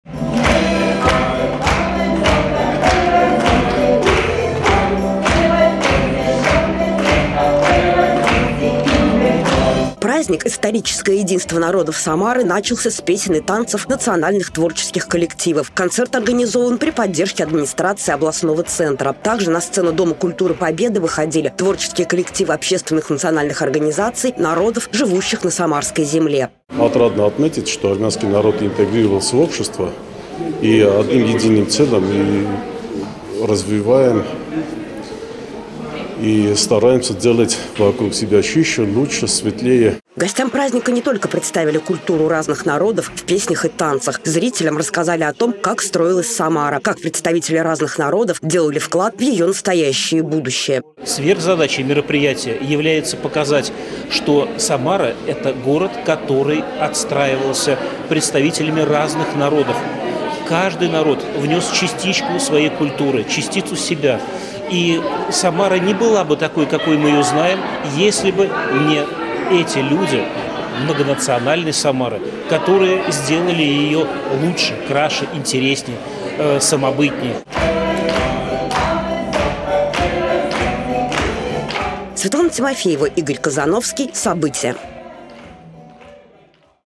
В Доме культуры "Победа" прошло мероприятие "Историческое единство народов Самары"
В Доме культуры "Победа" состоялся праздник "Историческое единство народов Самары". Национальные  общественные объединения подготовили концертную программу. Праздник был организован при поддержке администрации города и Дома дружбы народов. Подробности у корреспондентов "Событий".